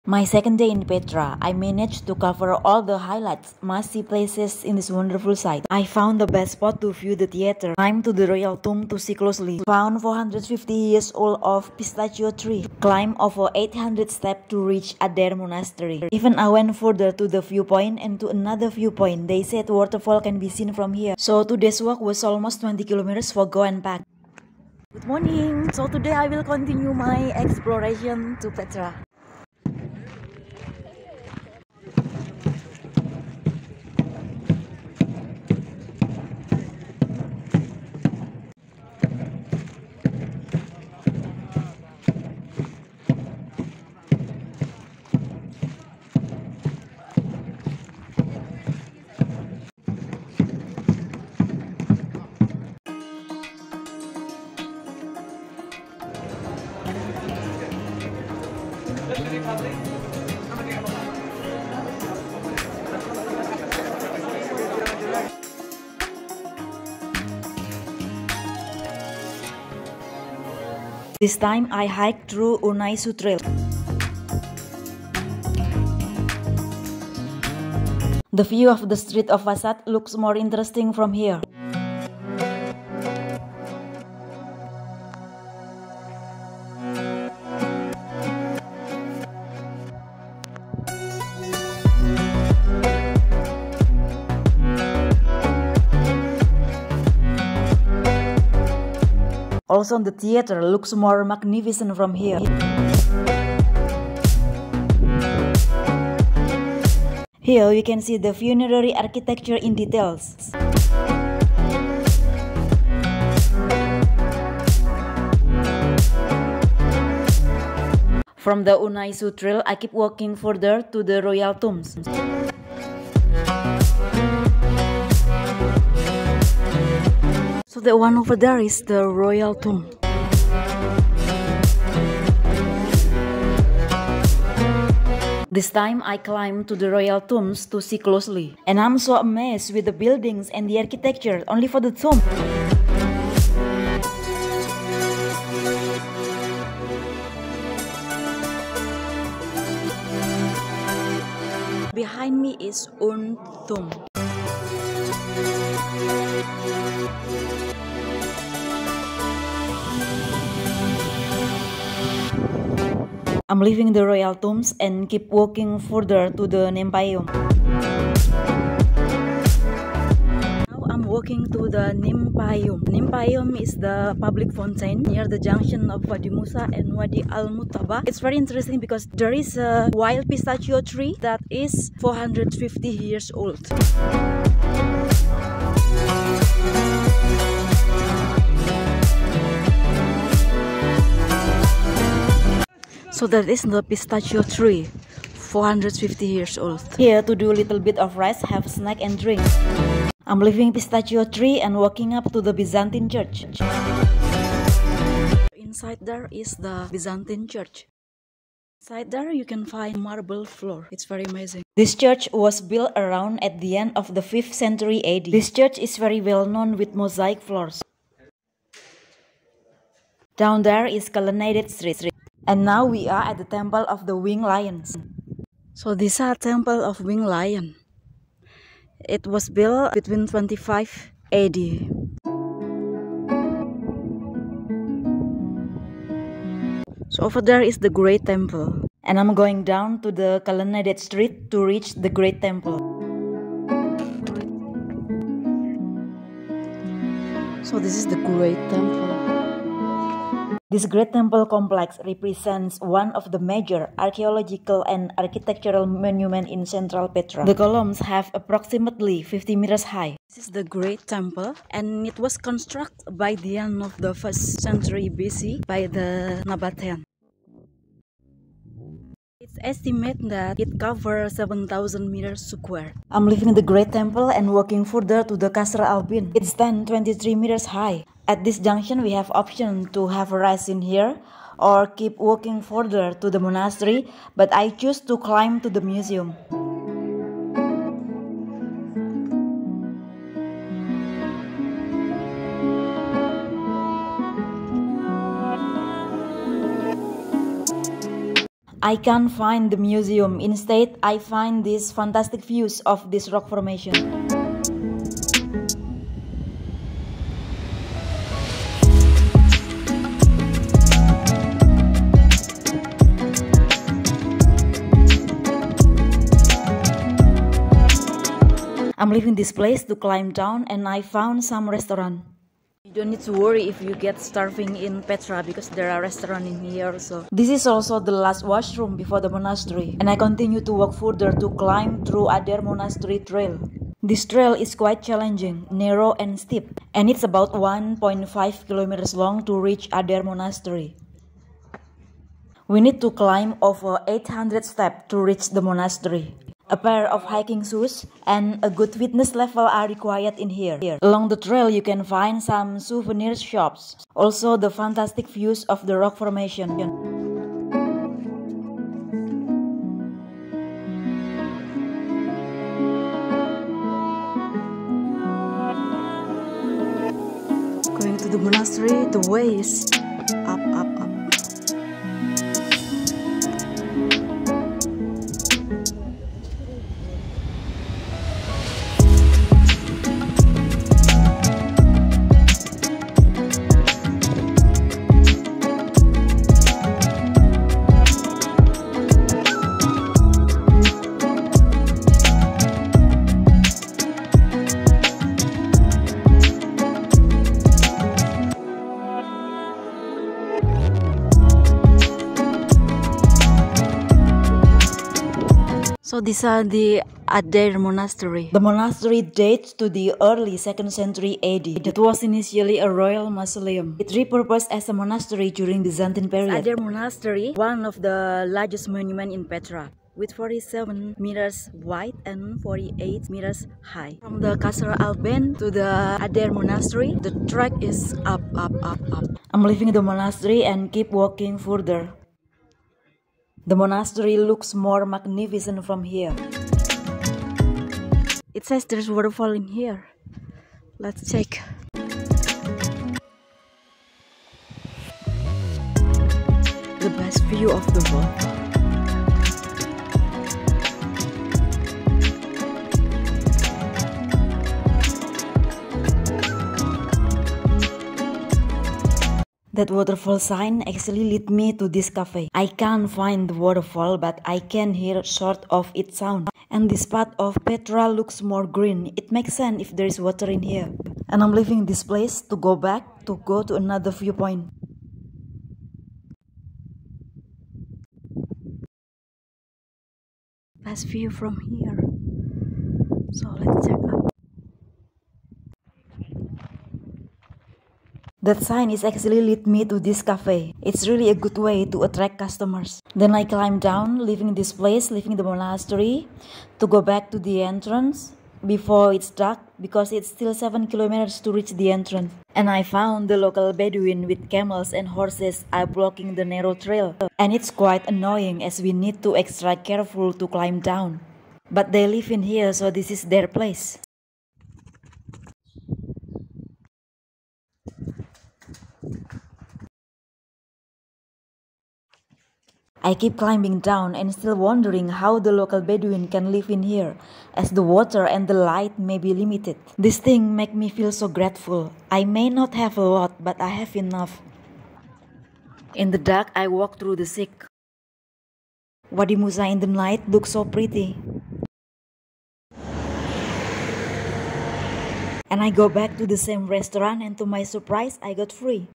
Second Day In Petra Jordan 🇯🇴: This World Wonder Keeps Me Amazed
My second day in Petra Jordan, I covered all the highlights must see places in this one of seven Wonders of the world. The more I went further the more I was amazed. This time I took the high route, started the hike from unayshu trail.
Here's my details journey :
00:00 intro
00:30 Guards show in Petra
01:14 Unayshu Trail & Tomb
01:40 The Theatre view
02:08 The Royal tombs
03:13 The Nymphaeum
03:24 Byzantine church
03:43 the temple of winged lions
03:57 Colonnaded street and the great temple
04:36 Qasr Al bint
04:45 Museum Trail
05:22 Basin Restaurant
05:34 Ad Deir Monastery Trail
06:33 the lion Triclinium
06:46 ad deir Monastery
07:10 viewpoints trail (waterfall)
08:22 Local Bedouin with horses
08:57 Wadi Musa in the night